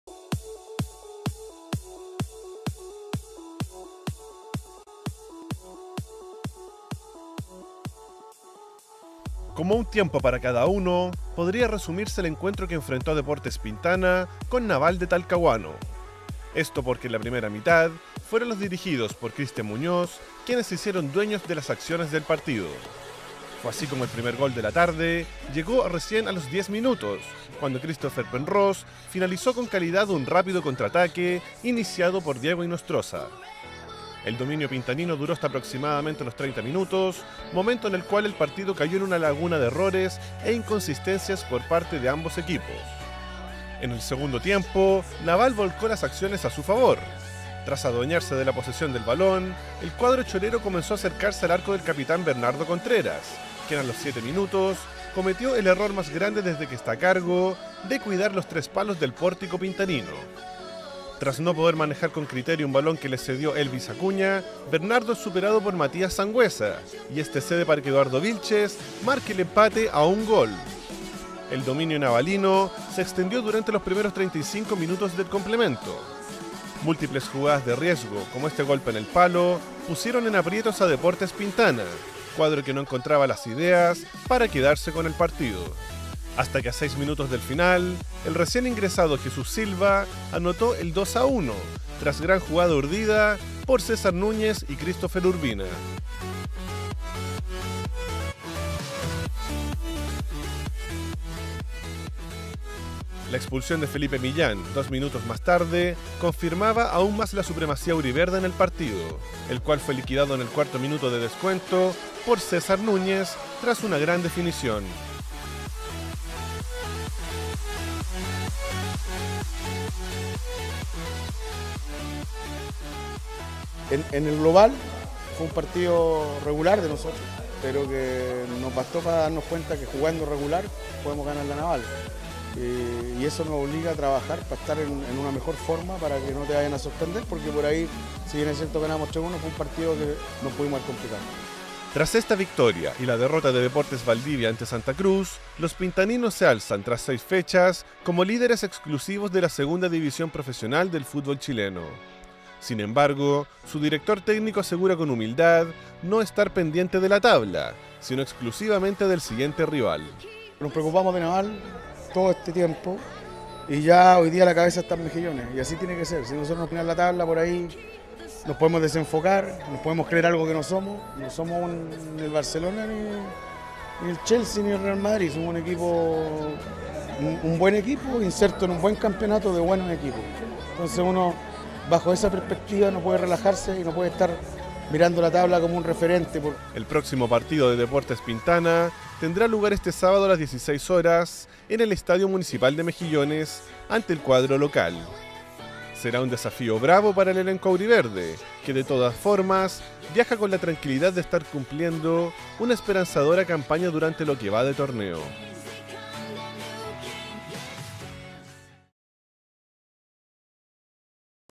Deportes Pintana vence a Naval y se mantiene al tope de la tabla
Una victoria por 3 goles a 1 consiguió Deportes Pintana sobre Naval de Talcahuano en calidad de local. El triunfo le permitió a los auriverdes consolidarse en lo más alto de la Segunda División Profesional.